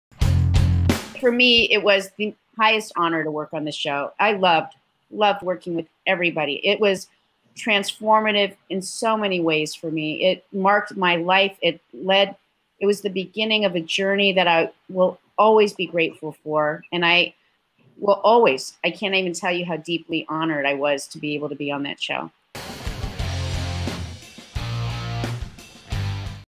Gabrielle Carteris On Being On 90210
What are Gabby's overall thoughts on the show?